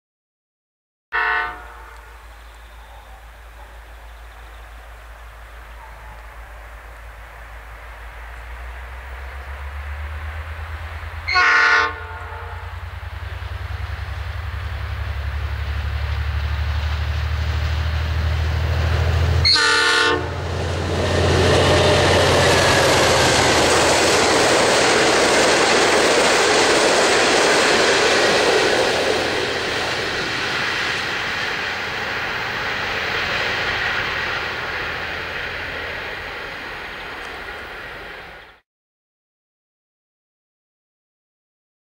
N451 at Willowite Road
N451 on its way to Warrnambool with the last train of the day on 12/2/2011. I had to run to get this film because we were walking there and left 5 mins late! Note the different horn on N451. No other N class has this horn set to my knowlege and it is only on one end hence I have only heard it's unique horn when it goes to Warrnambool.